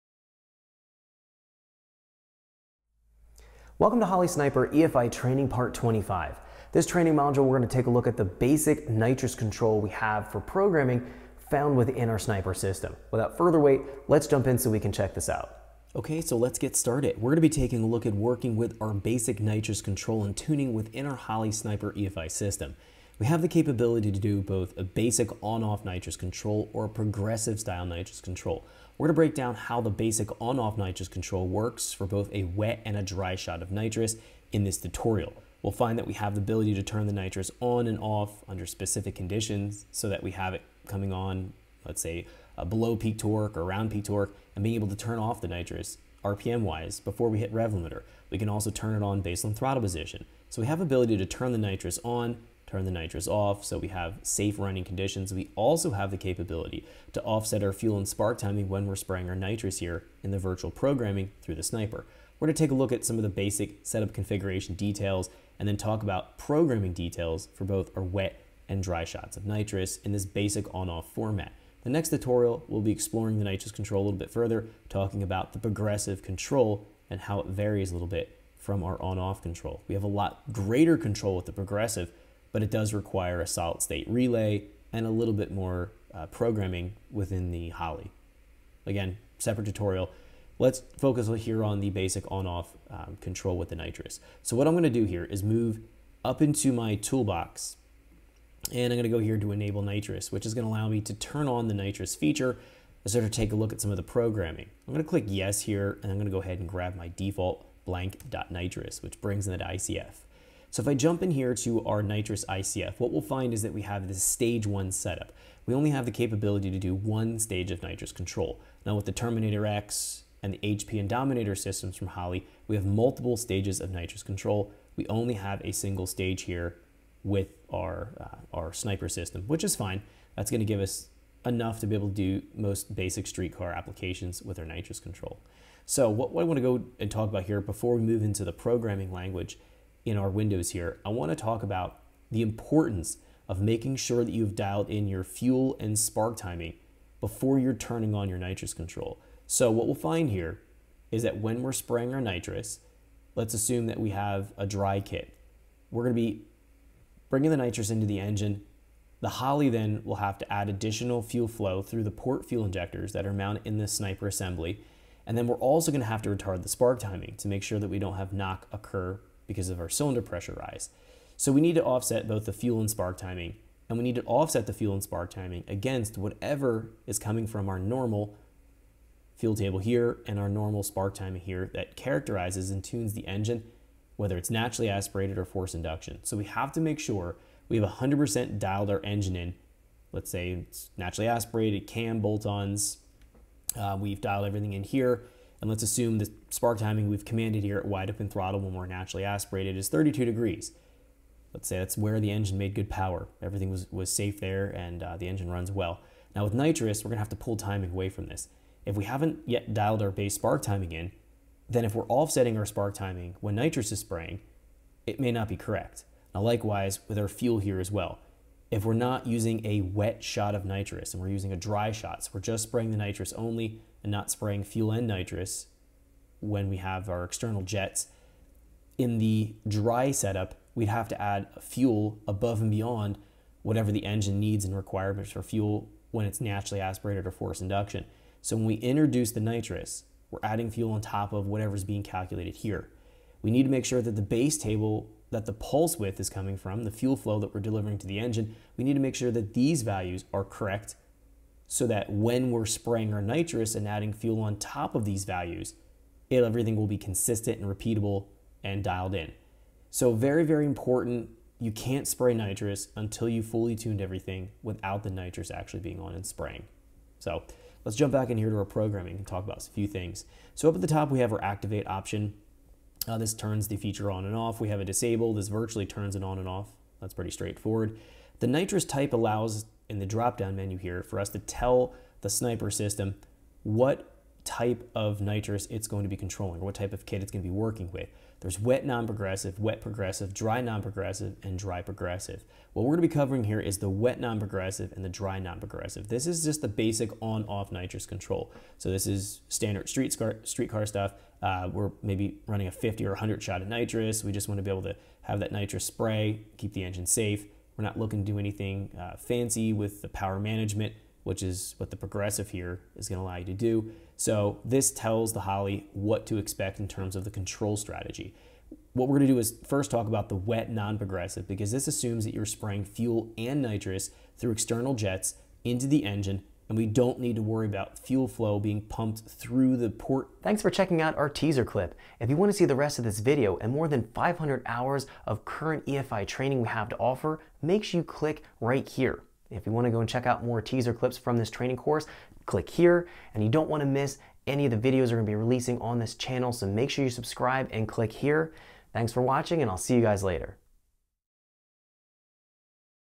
Holley Sniper EFI Training Part 25: Basic Nitrous Control | Evans Performance Academy
Holley Sniper EFI Training Part 25: Basic Nitrous Control walks you through how to set-up and tune a dry or wet non-progressive nitrous system with the Sniper system.